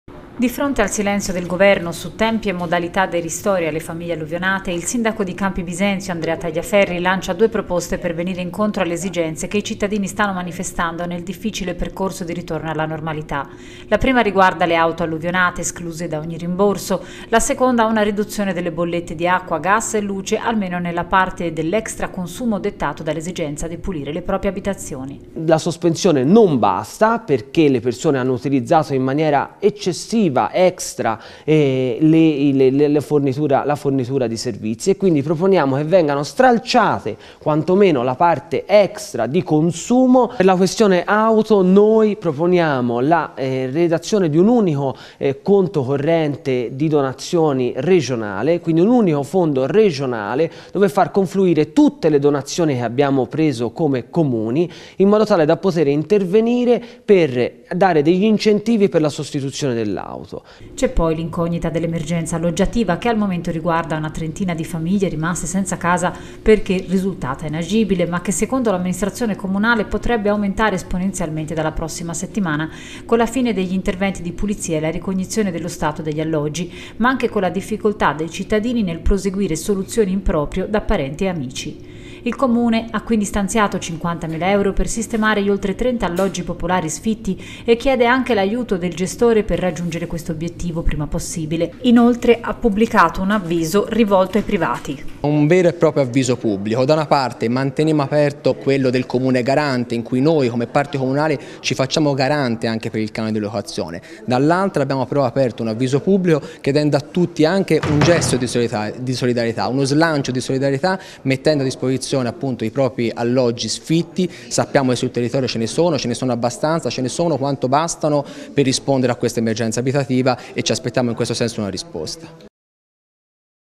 2023-11-22 CAMPI BISENZIO (FI) - ALLUVIONE, SINDACO, FONDO PER AUTO E SCONTI IN BOLLETTA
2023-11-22 CAMPI BISENZIO (FI) - ALLUVIONE, SINDACO, FONDO PER AUTO E SCONTI IN BOLLETTA